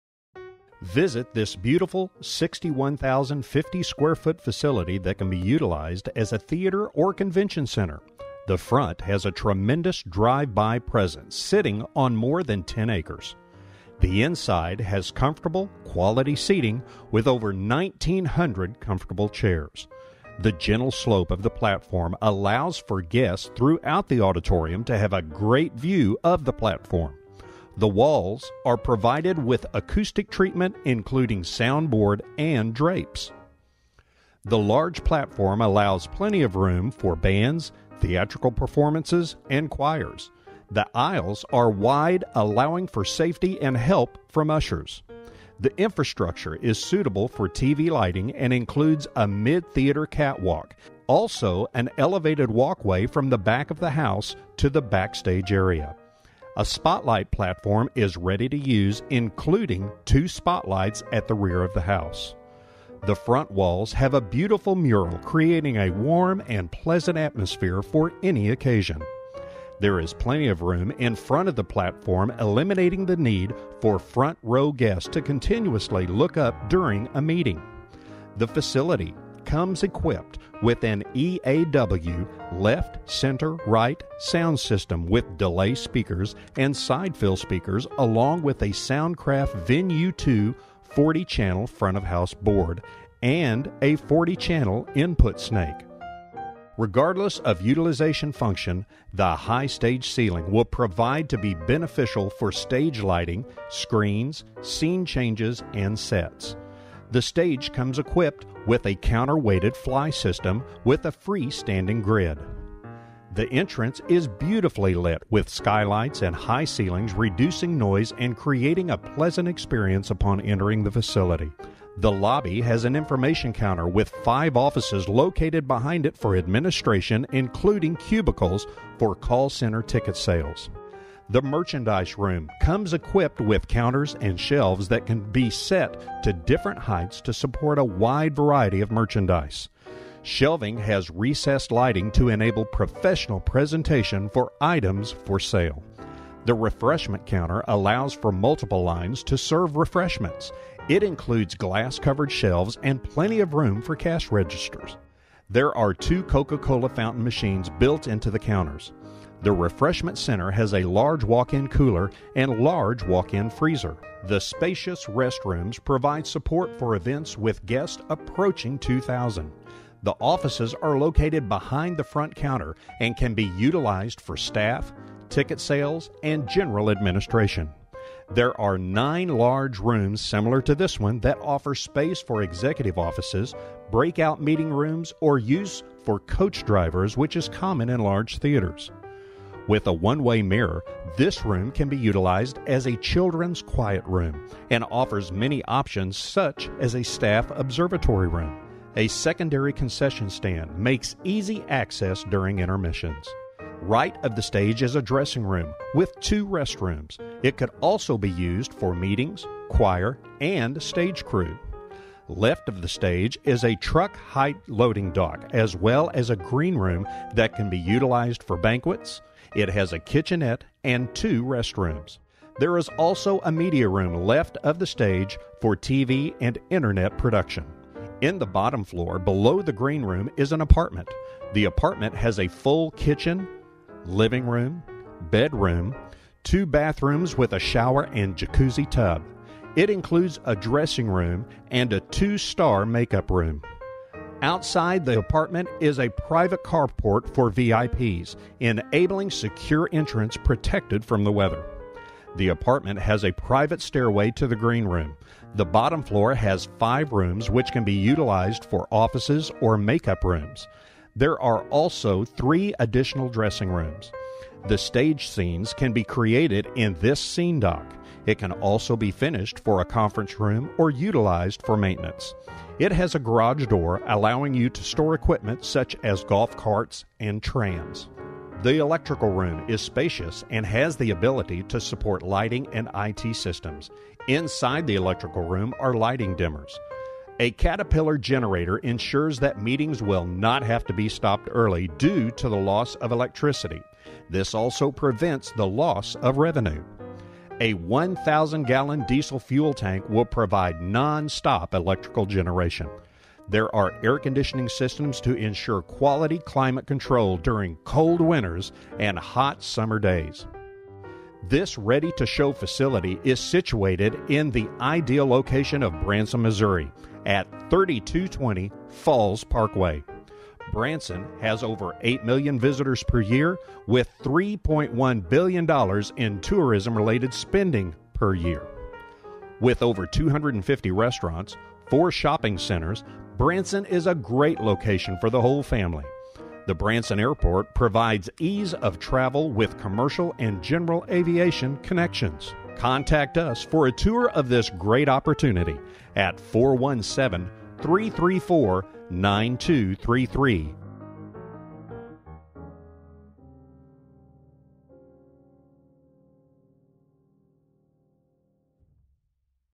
Church For Sale In Branson, MO
This beautiful 61,050 square foot theatre on 10.35 acres is for sale and immediate use in the heart of Branson, Missouri.  The short video provides an overview of some the theater including the stage, dressing rooms, scene dock, offices, private apartment with living quarters, lobby and retail shop. The theater seats approximately 2000.

The facility is great for theatrical, musical or performing arts events.

Situated in an ideal location just south of the HWY 76 strip, this theater has easy access to HWY 76 via 165 and is also easily accessible from Fall Creek Road. With multiple exit points to all major Branson roads, this theater is ideally situated for great traffic flow in and out of shows or events.

With an asking price of only $4,500,000.00 you cannot build a facility like this for $73.71 a square foot.

The theatre is ready for a show!